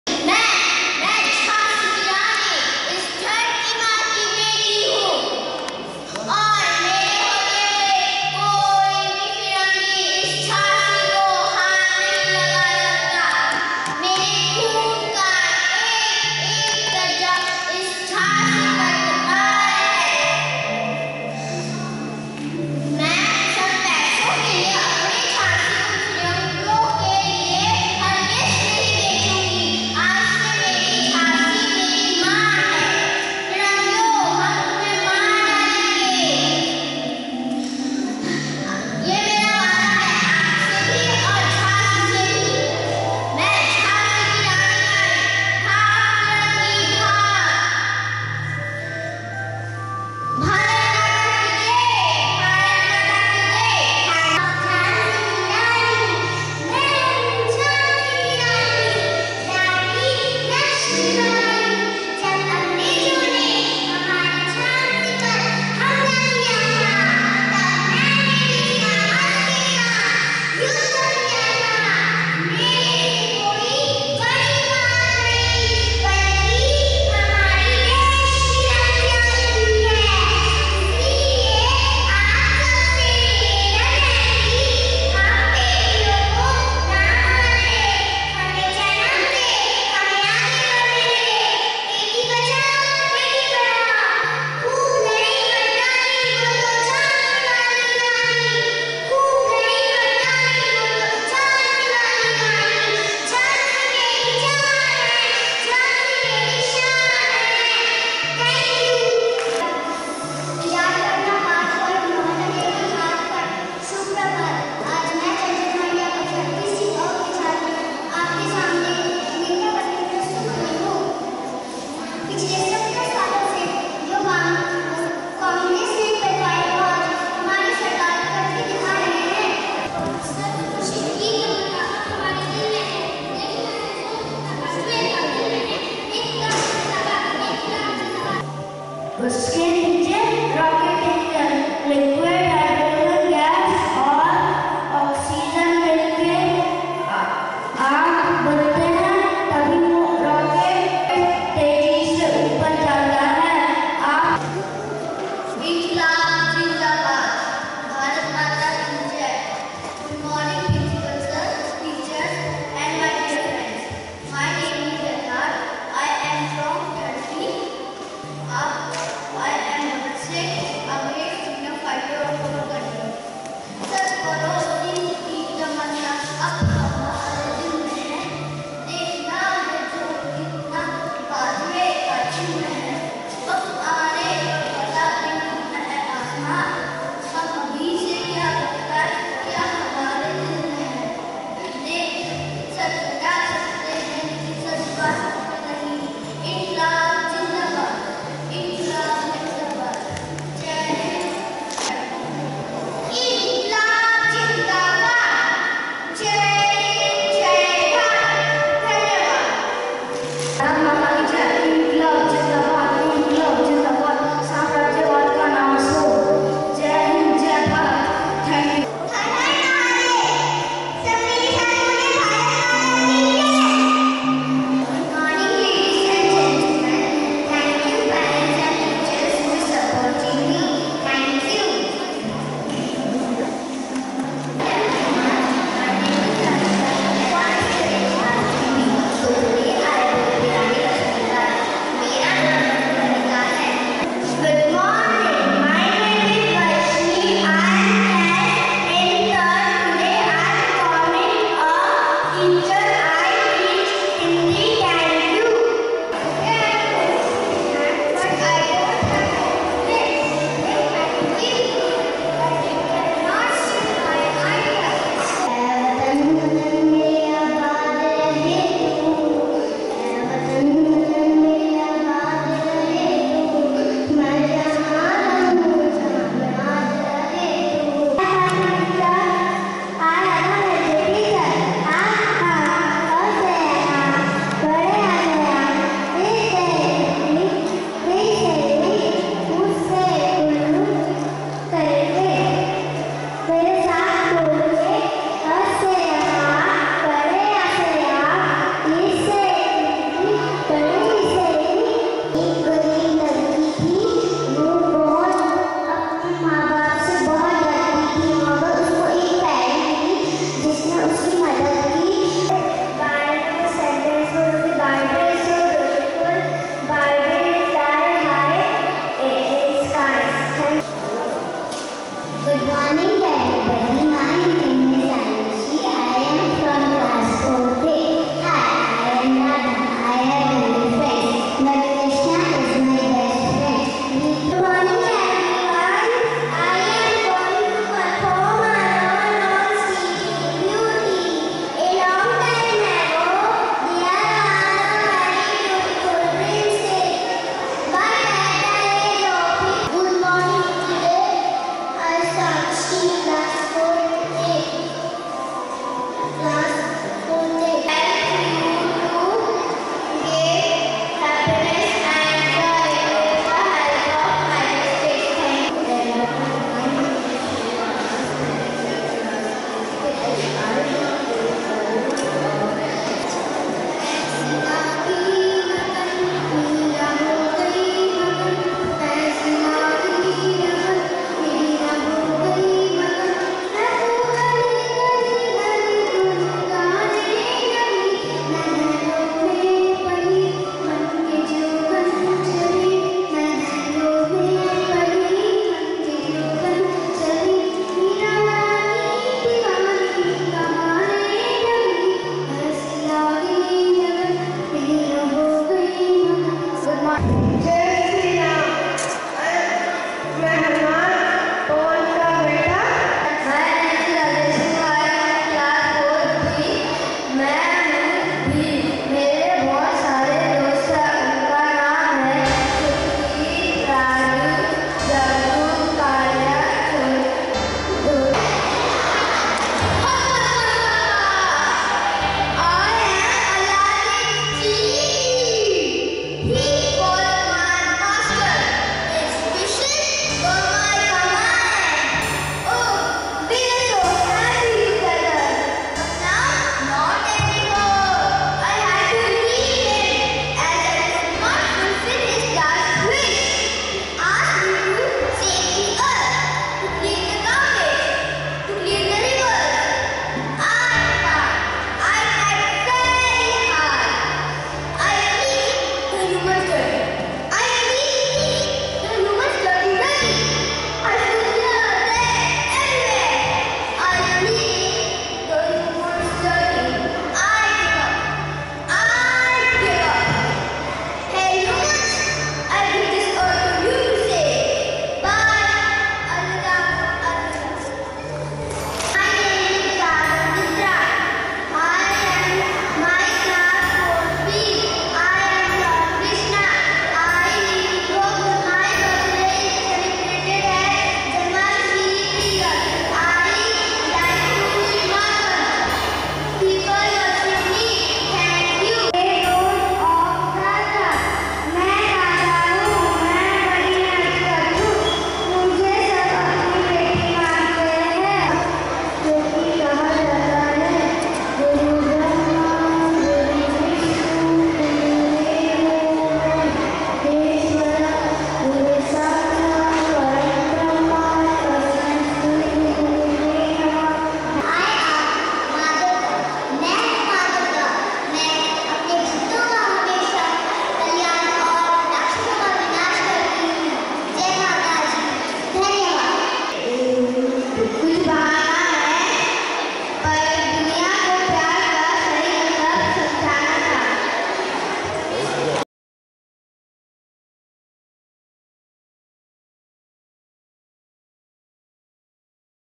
Fancy DRESS at APS Dinjan
Jr Students  (III-V)  of APS Dinjan participating in Role Play Activity !